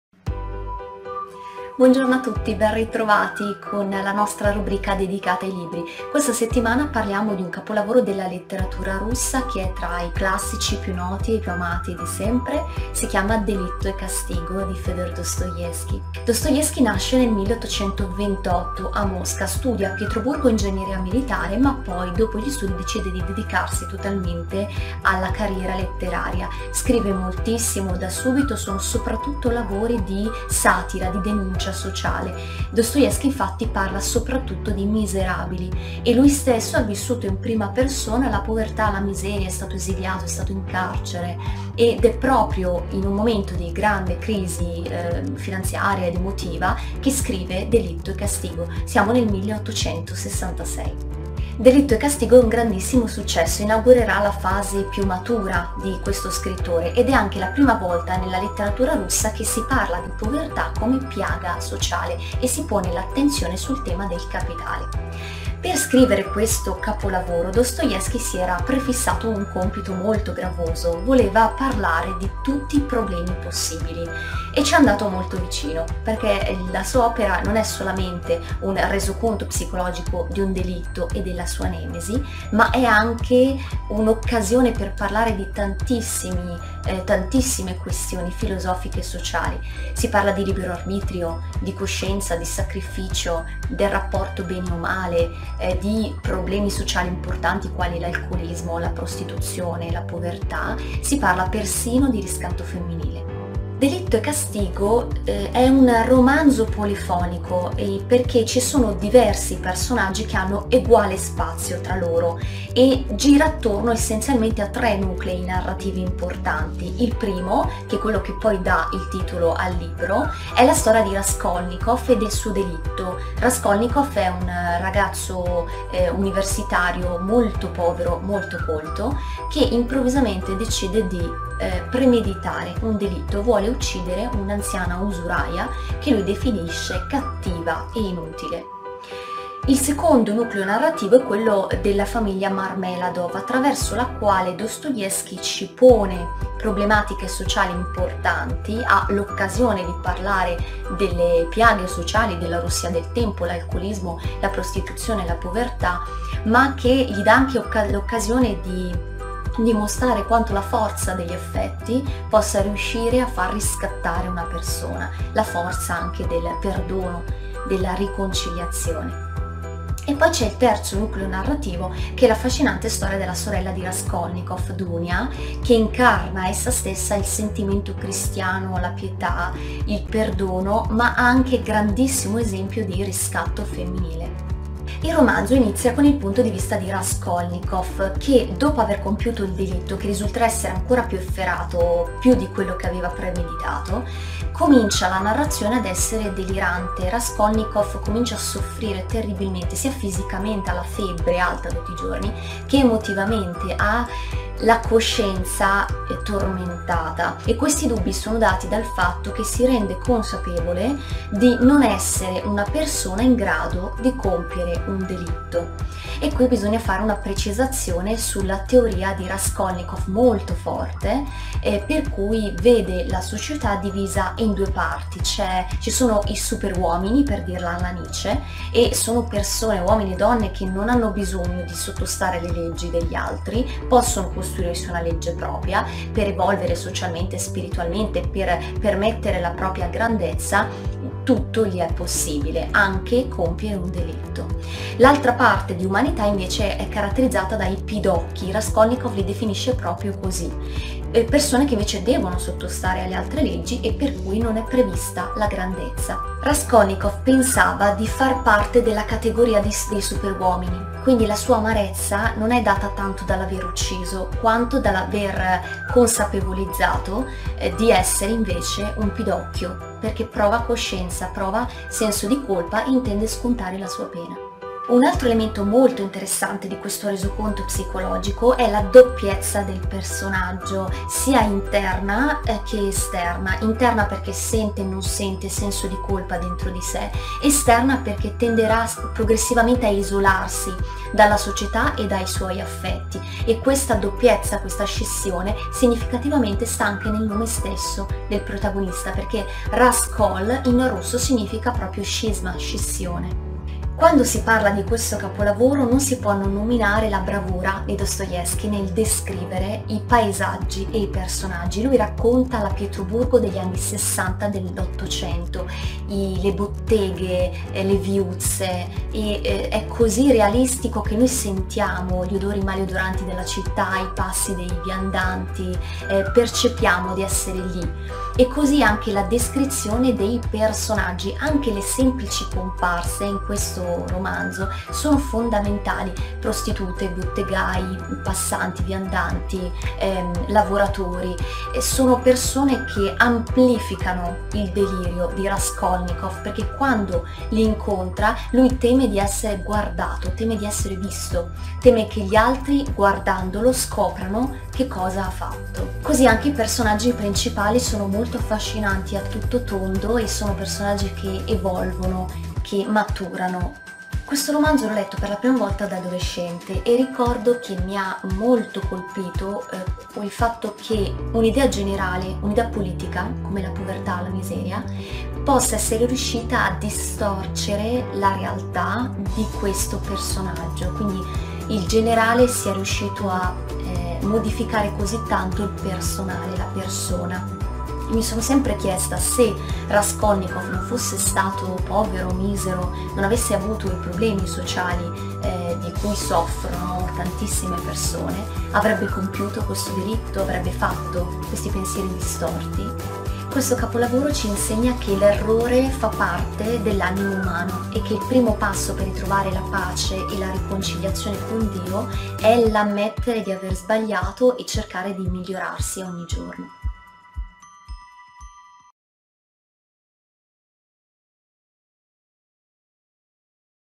consiglidilettura Delitto e Castigo di Fëdor Dostoevskij
In questo video Raffaella ci racconta brevemente il romanzo Delitto e Castigo di Fëdor Dostoevskij